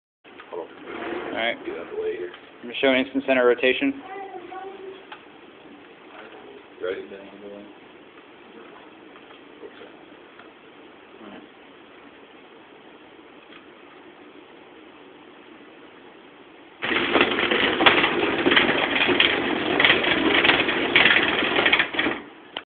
Robot Instant Center
Demonstrating the instant center of rotation of the robot. Made by Univ. of South Carolina mechanical engineering students.